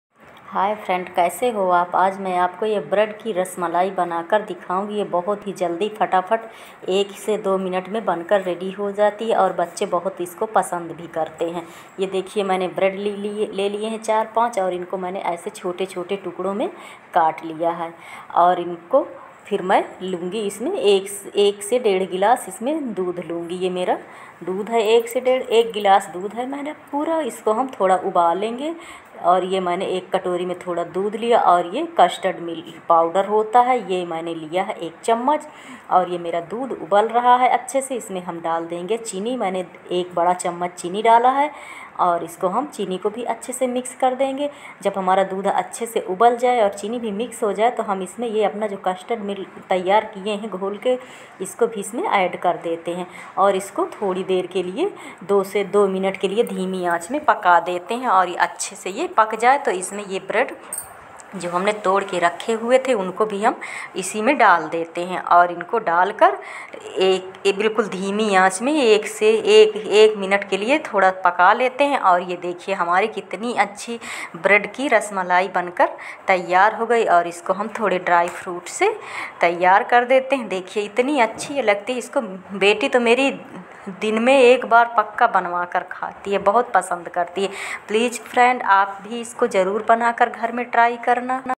jhatpat 2 minut mein banaen bread rasmalai
ekbarkhaogetoबार-बारkhaoge

bacche kya budhe bhi bachao sikhaenge

rasmalai banane ke liye ingredients

     8 bread lena hai unko aap kisi bhi size mein kat lena hai
dedh gilas dudh
100 gram chini
kuchh kate hue dry fruit
ek chammach custard powder kisi bhi company ka
dudh ko kadhai mein ham dalkar ek ubal aane Tak paka lenge
hamara dudh jab acchi tarah se Pak jaaye to usmein humne Jo custard powder liya hai usko thode milk mein mix karke dudh mein धीरे-धीरे dal dete Hain aur usko bhi acche se teen char wala neta paka lete Hain
dudh hamara acche se jab Tak Gaya FIR humko Apne Jo bread kat ke humne rakhe the unko धीरे-धीरे एक-एक karke dal dena hai
Gas ka flame humko bilkul Lo kar dena hai aur 1 minut Tak inko dudh mein pade Rahane dena hai
hamari rasmalai ekadam ready ho gai hai ismein ham upar se thode dry fruit kat kar dal dete Hain aur FIR do SE teen minut bad isko fridge mein rakh kar fridge kar dete Hain teen char ghanton ke liye
hamari rasmalai Bankar ekadam se thandi thandi ready ho gai hai aap isko ab sarv kar sakte hain

                please banakar jarur TRAI karna
                 thank you very much
please support me like my videos